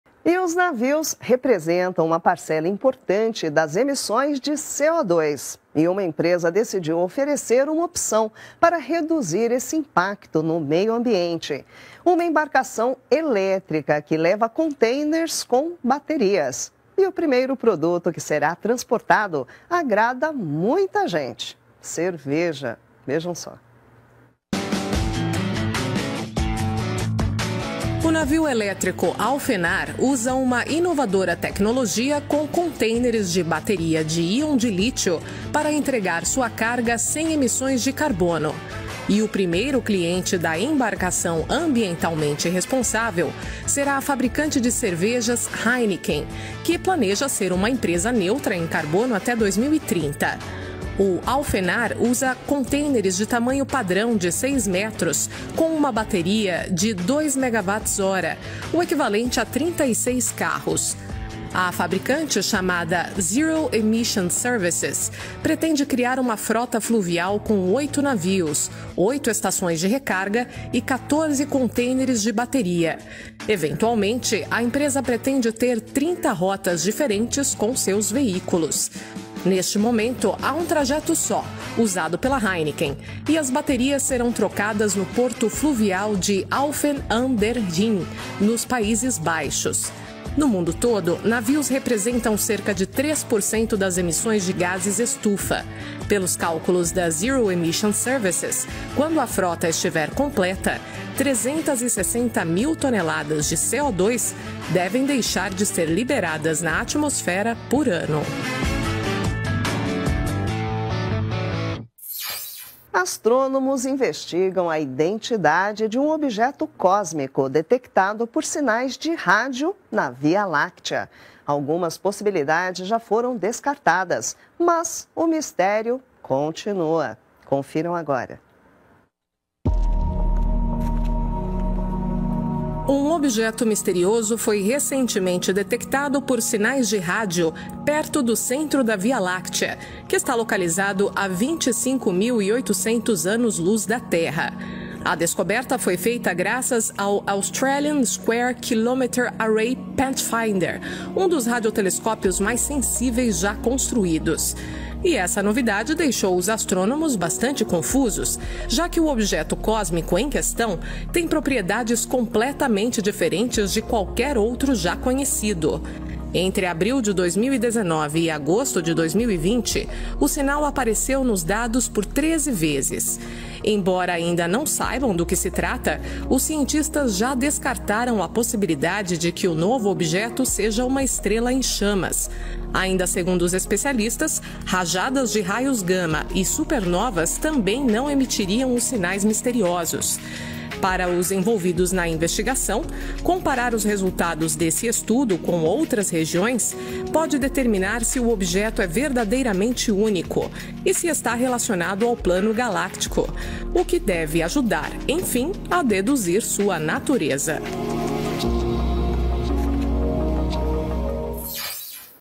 Cerveja ecológica? Navio elétrico usa contêineres com bateria para distribuir a bebida
Navios representam uma parcela importante das emissões de CO2. E uma empresa decidiu oferecer uma opção para reduzir esse impacto ao meio ambiente: uma embarcação elétrica que leva contêineres com baterias. E o 1º produto que será transportado é a cerveja!